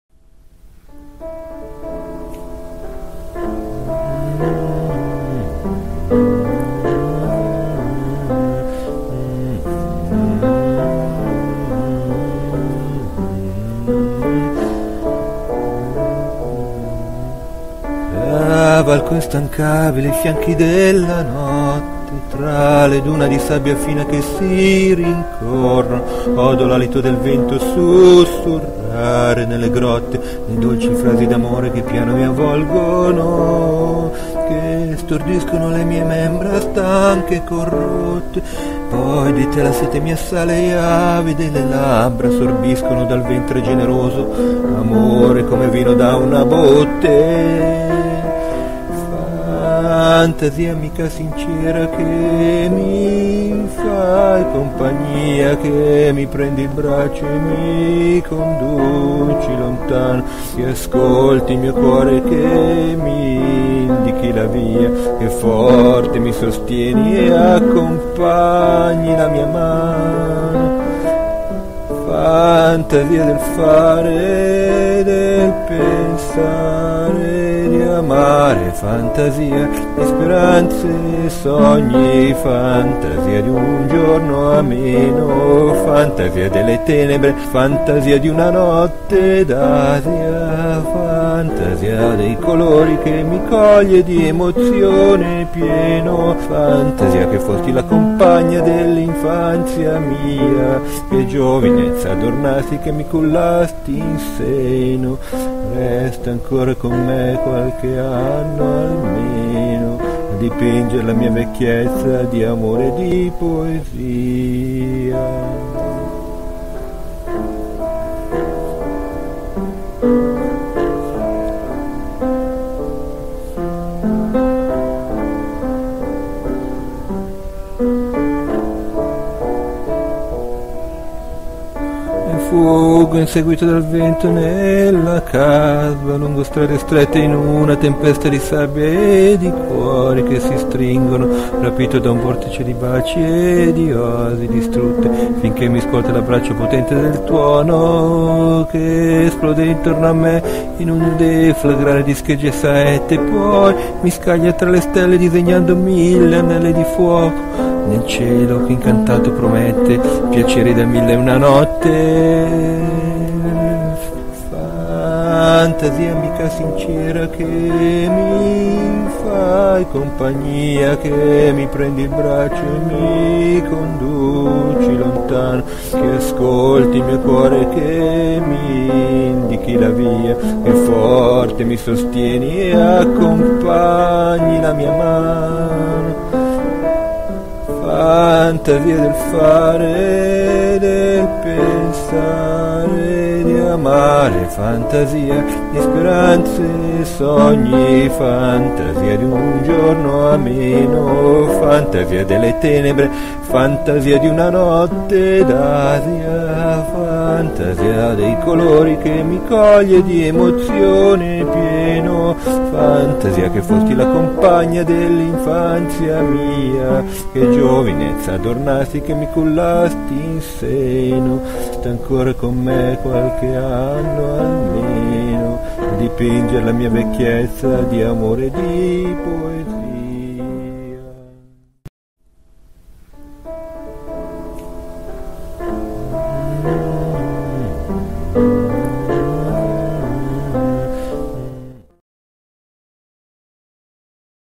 Fantasia
Cavalco instancabile i fianchi della notte,
tra le dune di sabbia fina che si rincorrono.
Odo l'alito del vento sussurrare nelle grotte,
le dolci frasi di amore che piano mi avvolgono,
che stordiscono le mie membra stanche e corrotte.
Poi di te la sete m'assale e avide labbra sorbiscono,
dal ventre generoso, amore, come vino da una botte.

Fantasia amica sincera che mi fai compagnia,
che mi prendi il braccio e mi conduci lontano,
che ascolti il mio cuore e che mi indichi la via,
che forte mi sostieni e accompagni la mia mano.
Fantasia del fare e del pensare, di amare, fantasia,
di speranze, di sogni fantasia, di un giorno ameno,
fantasia delle tenebre, fantasia di una notte d'Asia.
fantasia dei colori che mi cogli di emozioni pieno.
Fantasia che fosti la compagna dell'infanzia mia,
che giovinezza adornasti, che mi cullasti in seno,
resta ancora con me qualche anno almeno,
a dipingere la mia vecchiezza di amore e di poesia.

Fuggo inseguito dal vento nella kasba tra le strade strette
in una la tempesta di sabbia e di cuori che si stringono,
rapito da un vortice di mille anelli di fuoco e di oasi distrutte,
finchè mi scuote l'abbraccio prepotente del tuono
poi mi scaglia tra le stelle in un deflagrare di scheggie e saette
nel cielo che incantato promette piaceri da mille e una notte

fantasia di speranze e sogni, fantasia di un giorno ameno,
a dipingere il tramonto  di amore e di poesia.